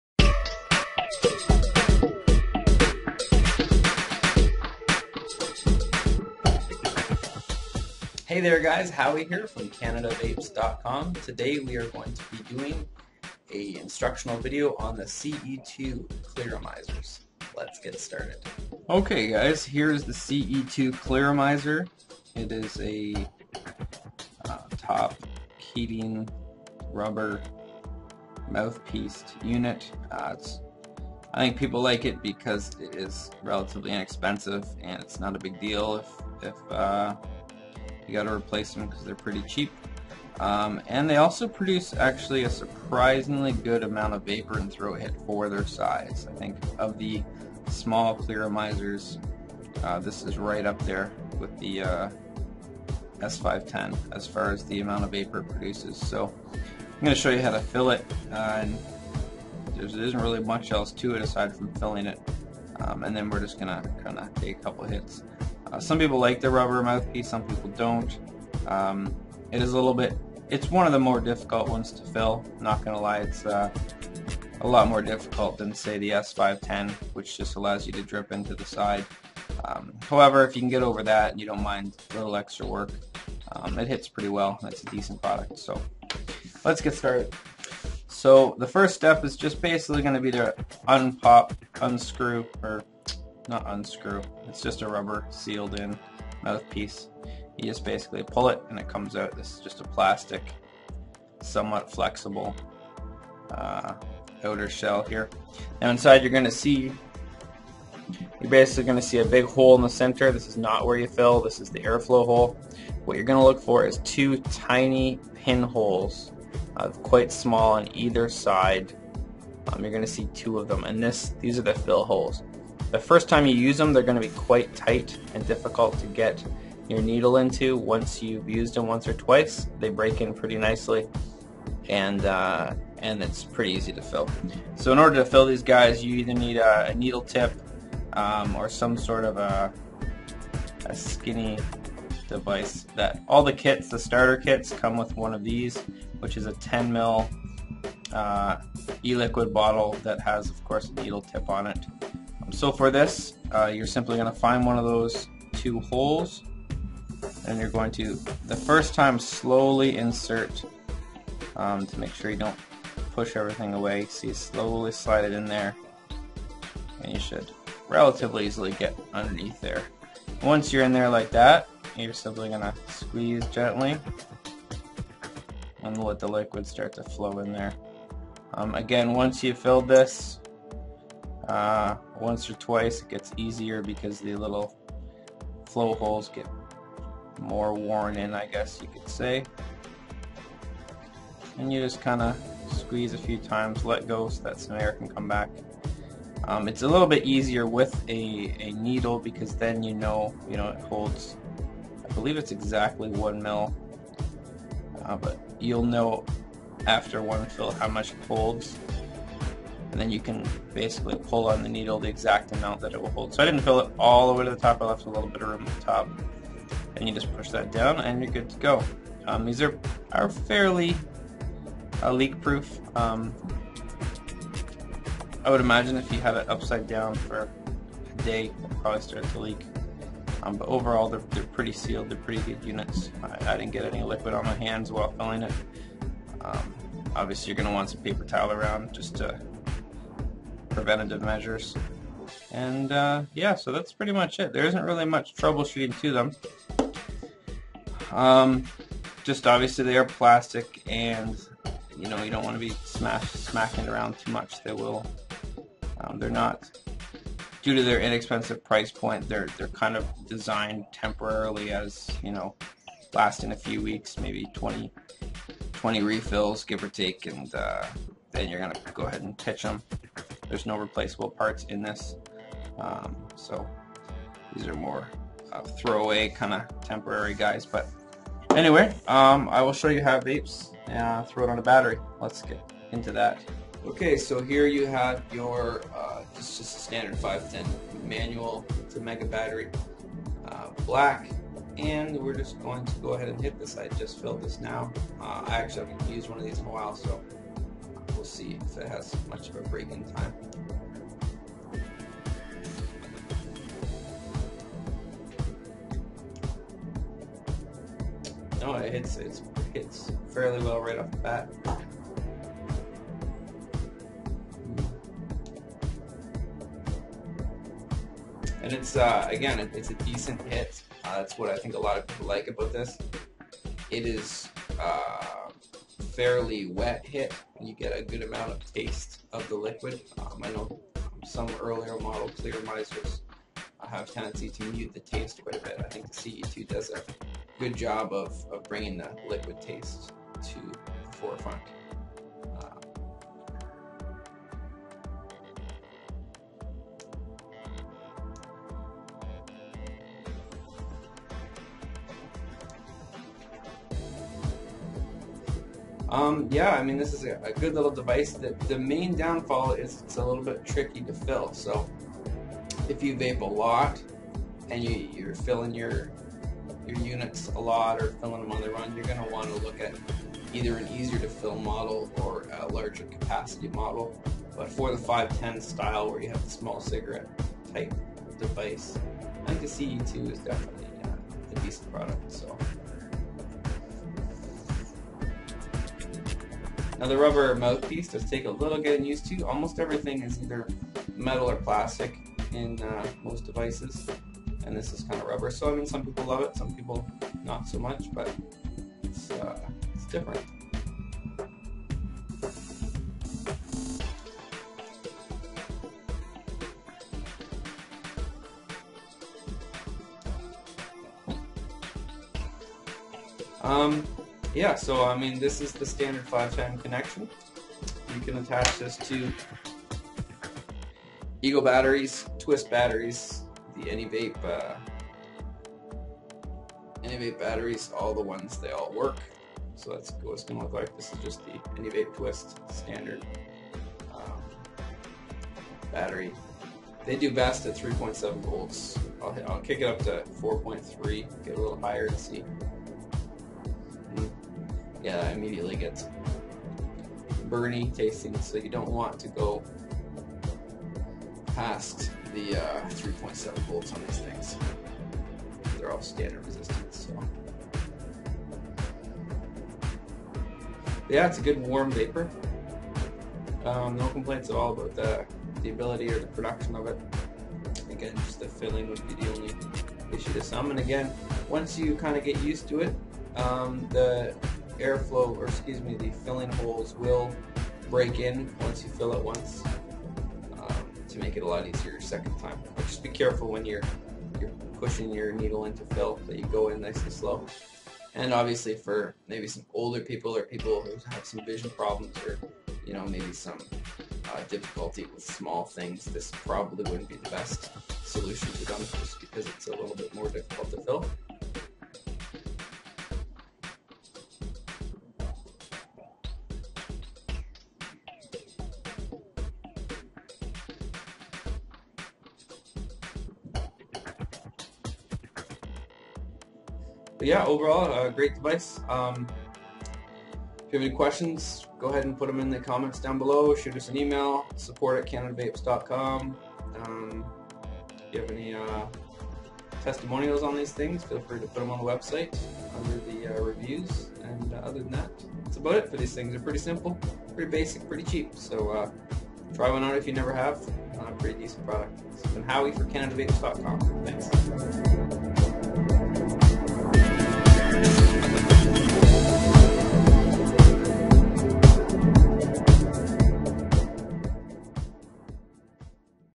CE2 clearomizer instructions, filling, tips, and vaping with the CE2 clearomizer
Howie shows you how to fill and use the CE2 clearomizer.